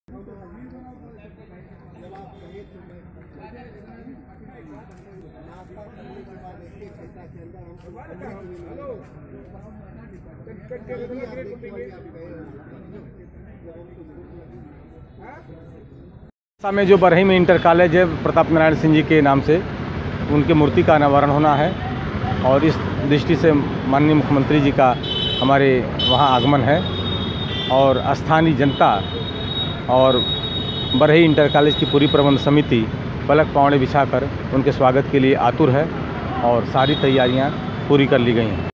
गोरखपुर: बाबू प्रताप नारायण सिंह की प्रतिमा का अनावरण करेंगे CM योगी आदित्यनाथ
गोरखपुर: ग्रामीण क्षेत्र में शिक्षा की अलख जगाने वाले बाबू प्रताप नारायण सिंह की प्रतिमा का लोकापर्ण करने बुधवार को मुख्यमंत्री योगी आदित्यनाथ गोरखपुर आएंगे. सीएम के आगमन को लेकर प्रशासन तैयारियों में जुट गया है.